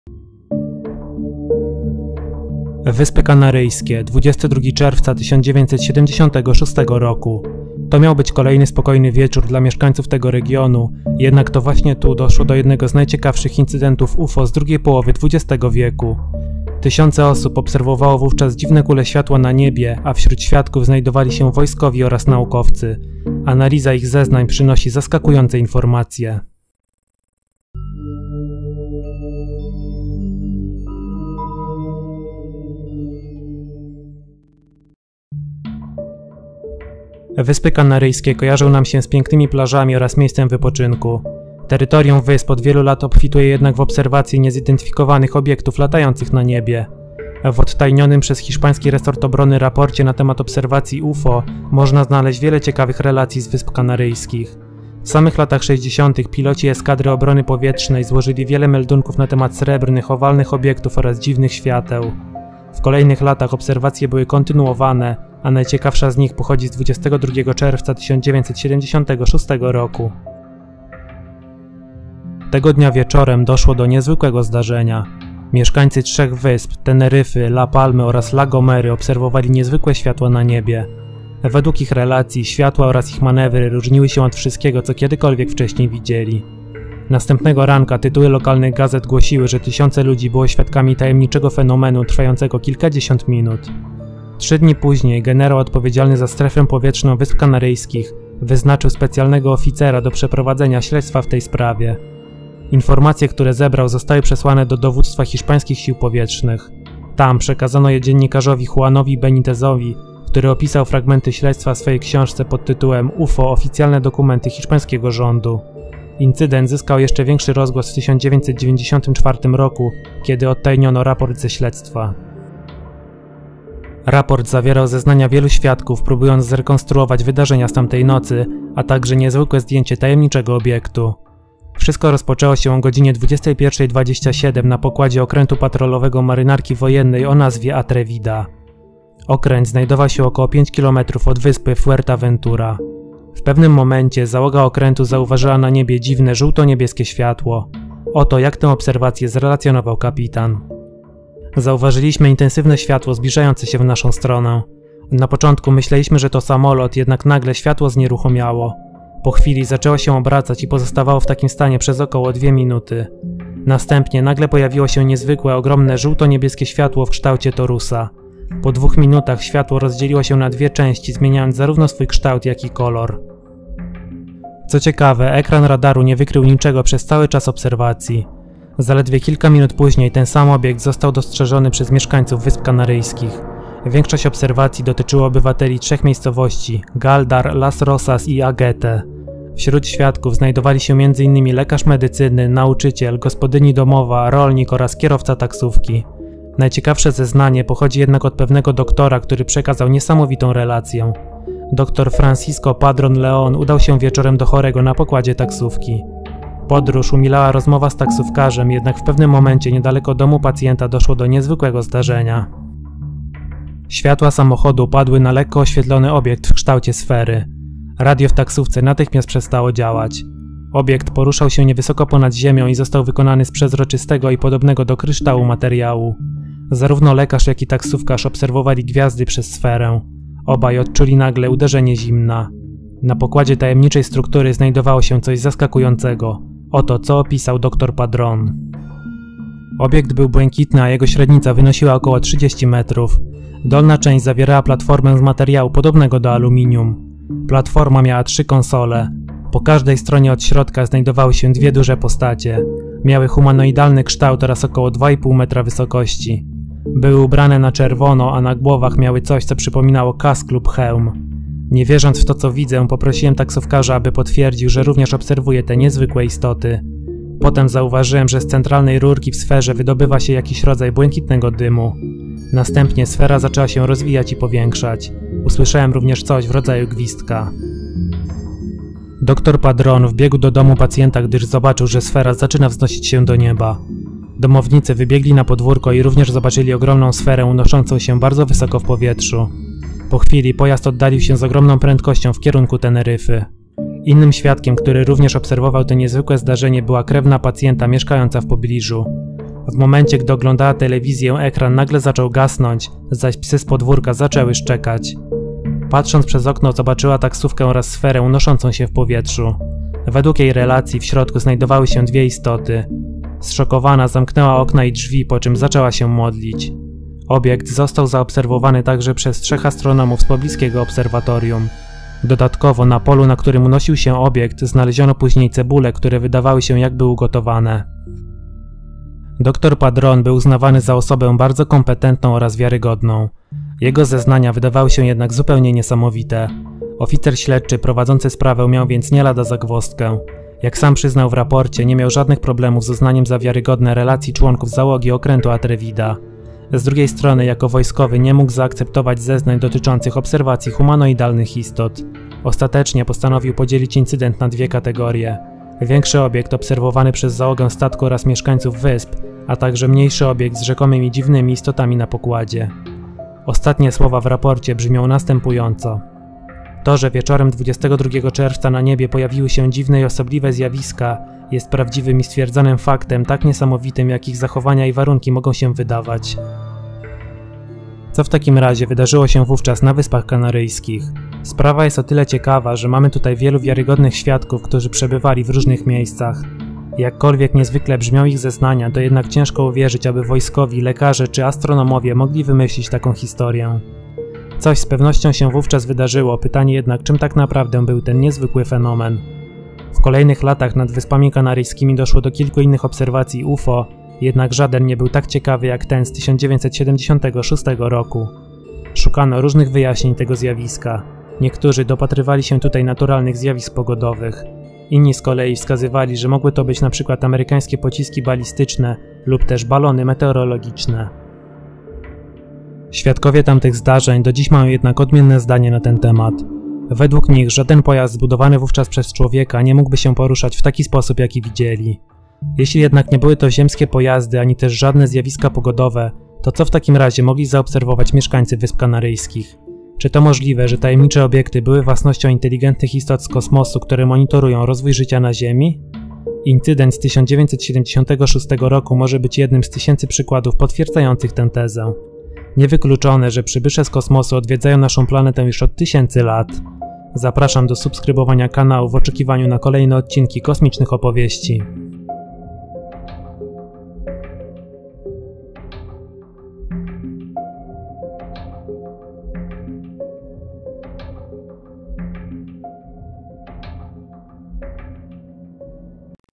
Incydent UFO na Wyspach Kanaryjskich w 1976 roku
UFO i kosmici na Wyspach Kanaryjskich? 22 czerwca 1976 roku doszło tam do ciekawego incydentu, który do dziś pozostaje tajemnicą. Tysiące osób obserwowało wieczorem dziwne kule światła na niebie. Wśród świadków znajdowali się nie tylko przeciętni obywatele, ale także wojskowi czy naukowcy. Zagadkowe obiekty obserwowali m.in. członkowie załogi okrętu Atrevida. Kilka osób, w tym renomonowany doktor, opisywało także relacje z bliskiego spotkania z dziwnymi istotami, które miały się znajdować na pokładzie tajemniczych obiektów. Sprawą zajmowali się przedstawiciele hiszpańskich sił powietrznych, jednak wg ich raportu trudno o racjonalne wytłumaczenie tego incydentu, który do dziś pozostaje niewyjaśniony.